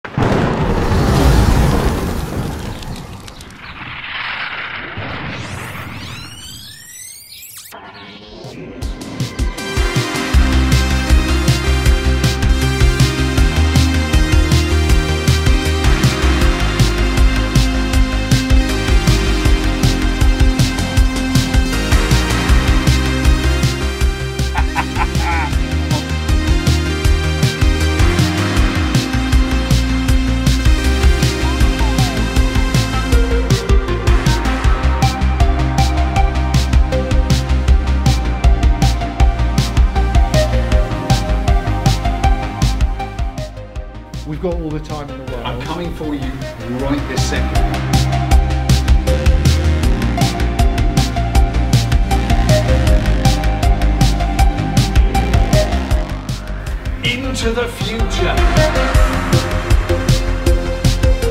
Making of our Back to the Future themed global event
Some highlights from the making of Back to the Future Themed event content.

Carswell Gould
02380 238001